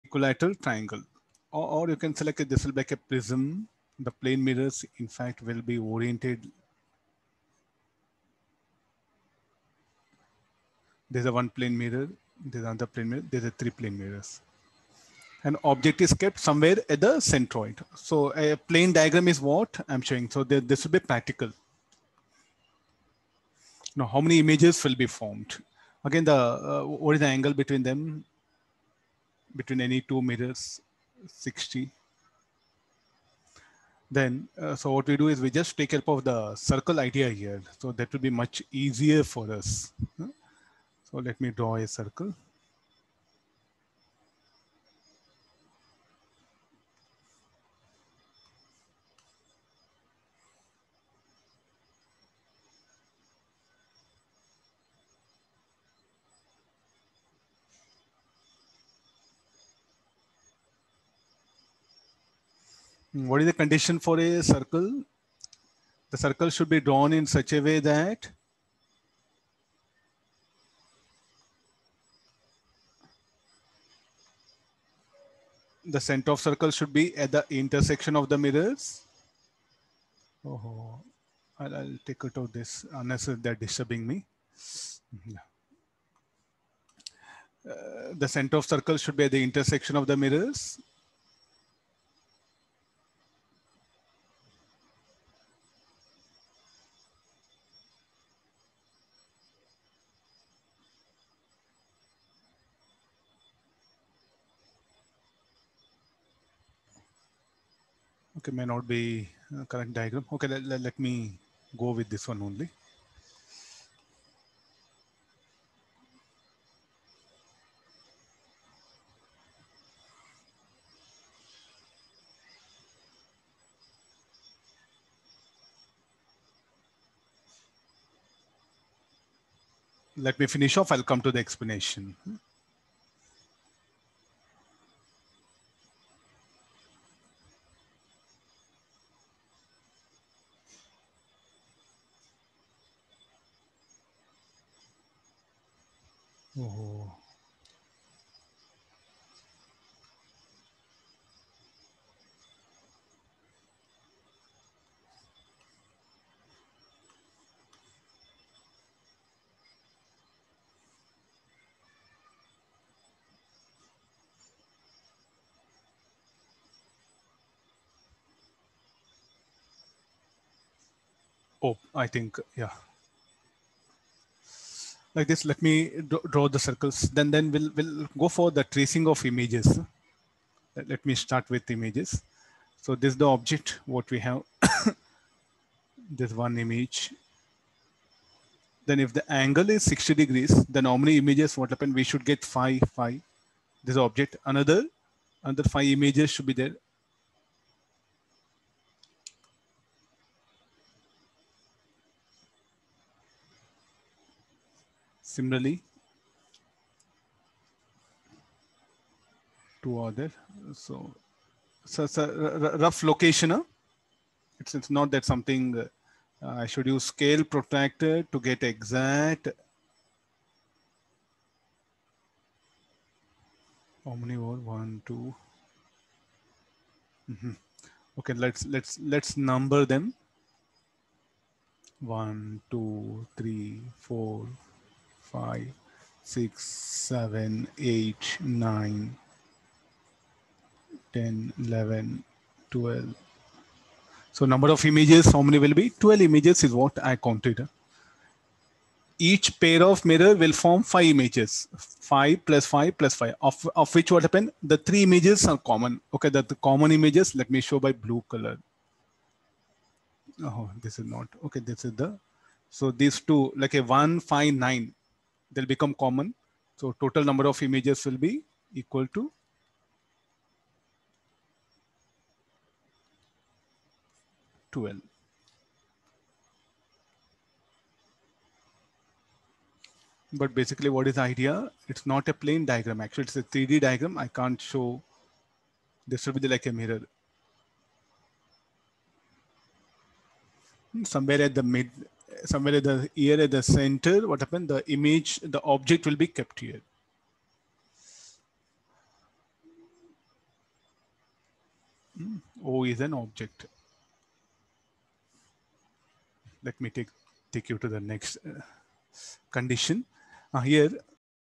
36) Ray Optics Physics | Number of images formed by three plane mirrors , with object lying at
the centroid of triangular reflecting mirrors | Ray Optics class 12 physics | physicsnumericalshelp sridharsirphysics jeeadvancedphysics iitjeephysics jeeadvanced Thanx for watching this video😊 . Experience--20yrs ; Mentored below 100 and 1000 JEE Advanced Ranks.